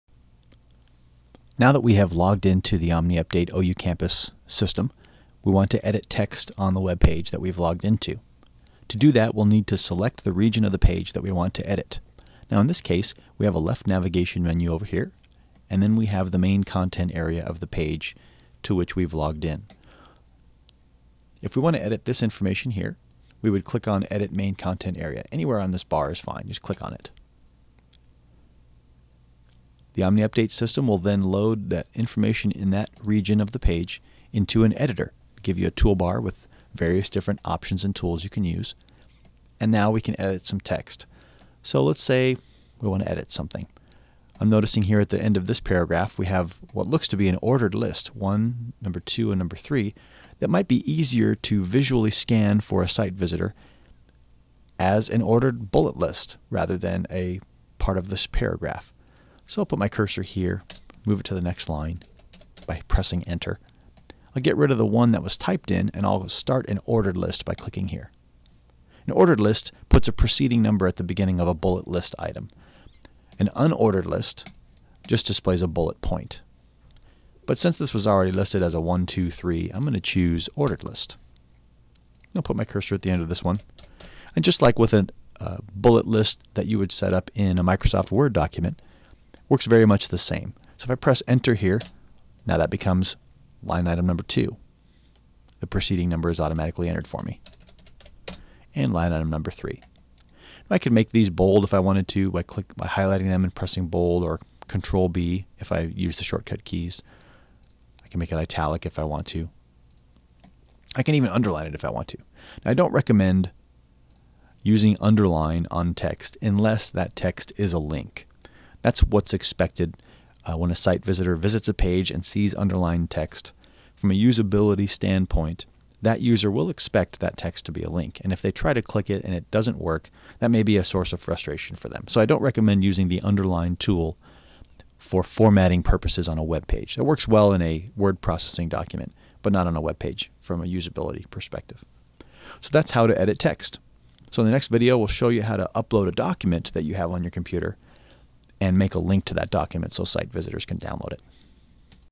VVC - How-To - OU Campus - Editing Text
This video demonstrates how to edit text using the OU Campus web CMS.